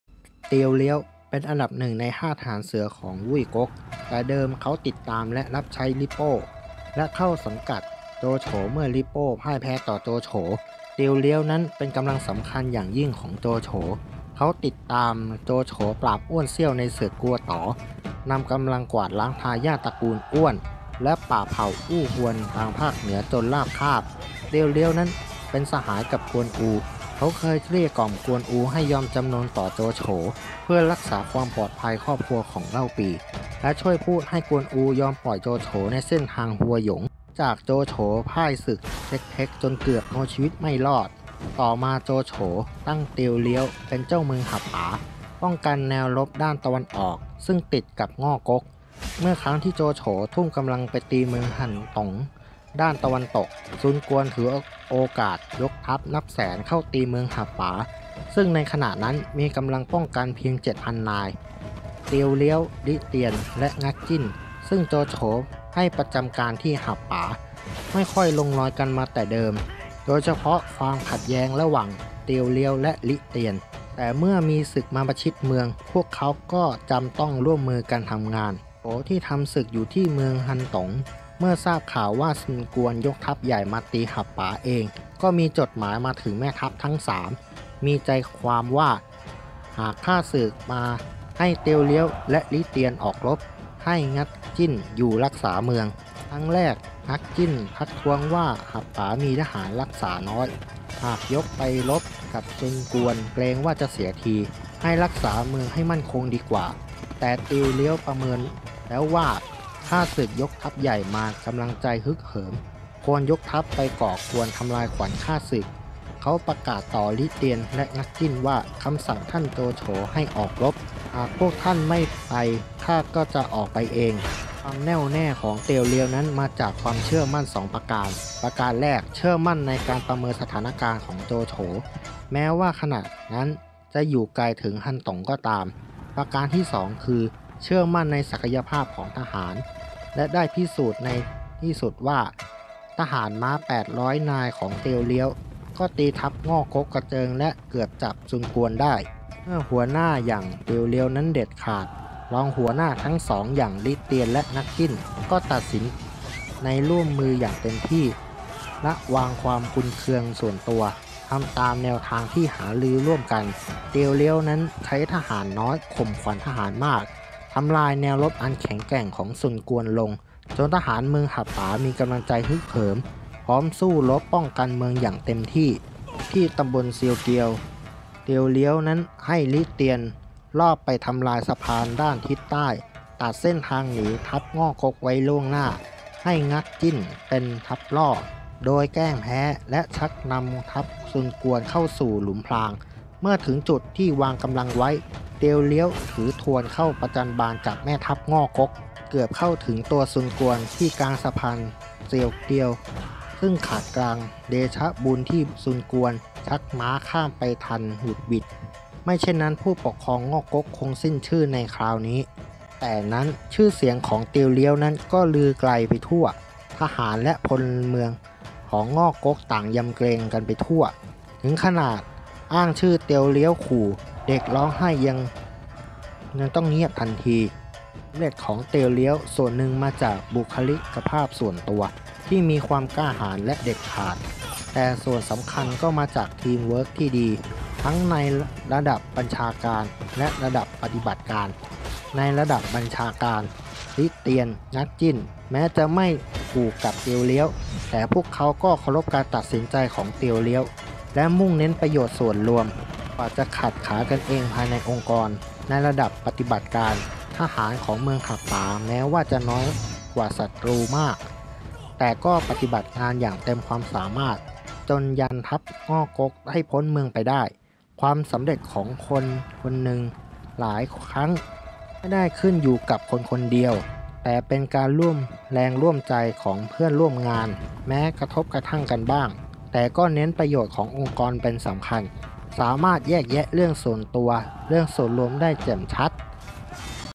สามก๊ก เตียวเลี้ยว: ยอดขุนพลแห่งวุยก๊ก By Zentrefheart
สร้างชื่ออย่างเตียวเลี้ยว : ละวางความขัดแย้ง ร่วมแรงเพื่อส่วนรวม
คลิปนี้เป็นการวิเคราะห์ตัวละครในยุคสามก๊กเท่านั้น
หากมีข้อผิดพลาดประการใด ต้องกล่าวขออภัยมา ณ ที่นี้ด้วย